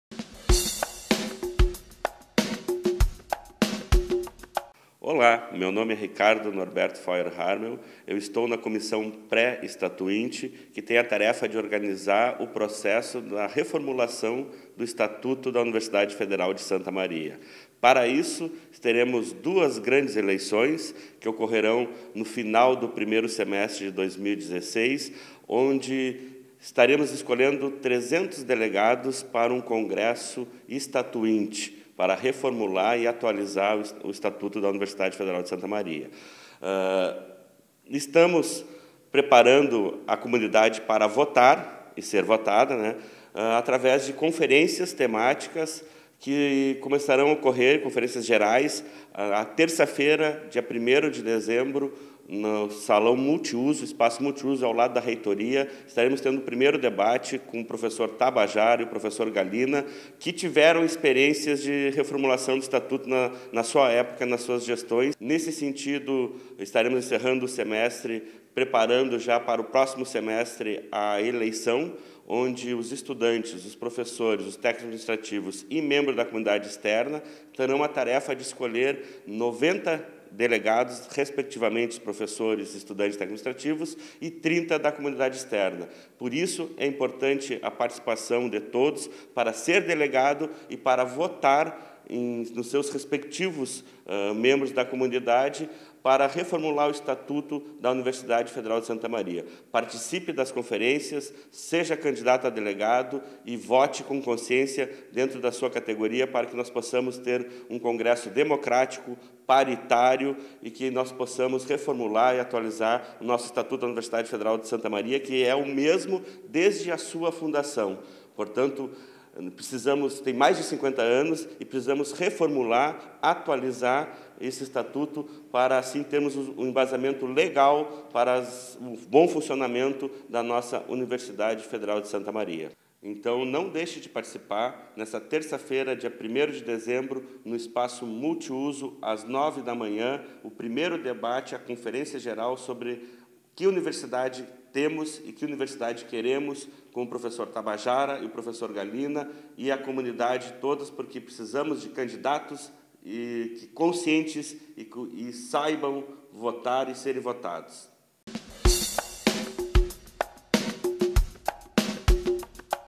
UFSM em Ação - Debate "Estatuinte: por quê? Você conhece a universidade que temos?"
O estatuto da UFSM, documento de organiza e rege a da universidade, é o mesmo desde a sua criação. Para que ele seja reformulado e atualizado, a instituição passa por um processo estatuinte.
Dentro da programação, está incluído o debate "Estatuinte: por quê? Você conhece a universidade que temos?", que acontece na próxima terça (1ª de dezembro), às 9h no Espaço Multiuso, ao lado do prédio da Reitoria.